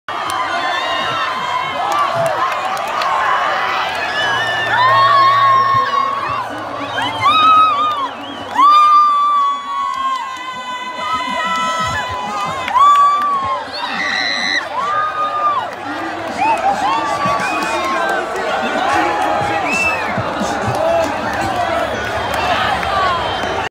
La joie des Croates à 2-1
Ambiance au Glacis, à Luxembourg, pour le match Croatie-Angleterre.